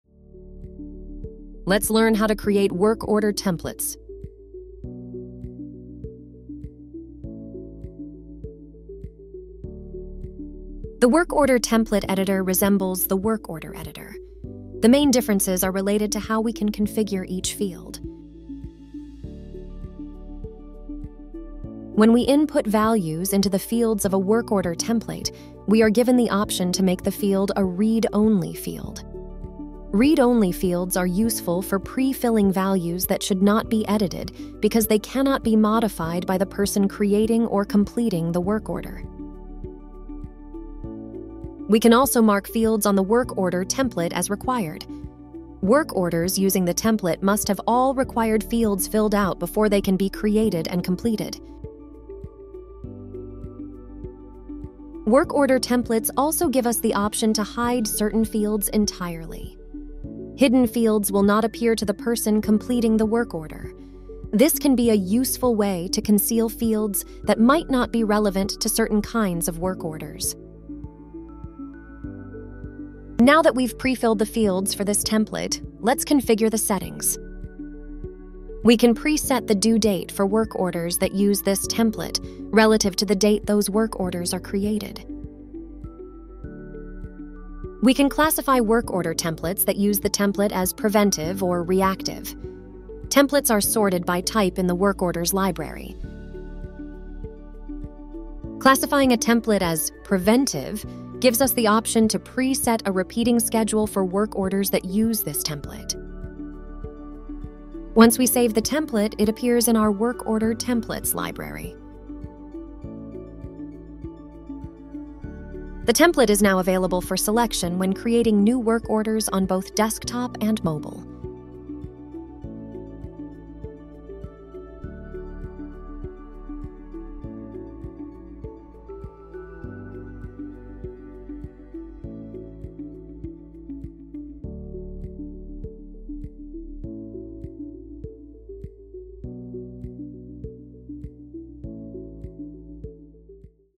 Navigate MaintainX | Creating Work Order Templates
Learn how to create work order templates and select templates when creating work orders.

Download MaintainX from the App Store and Google Play for free and see why our app is the top-rated maintenance management app today!